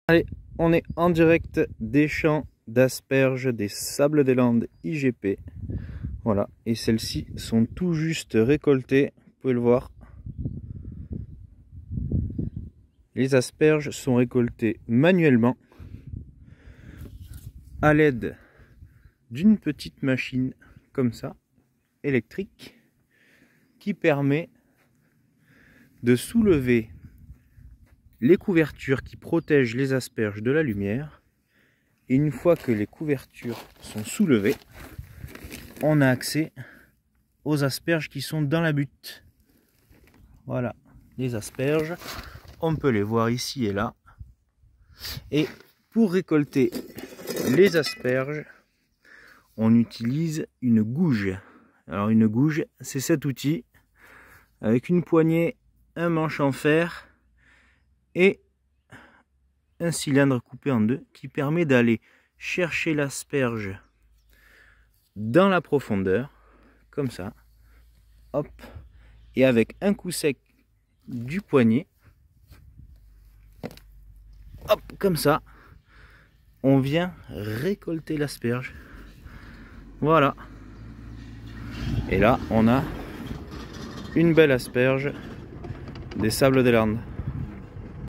La récolte des Asperges des Sables des Landes
Je t'explique la récolte des Asperges des Sables des Landes IGP et Label Rouge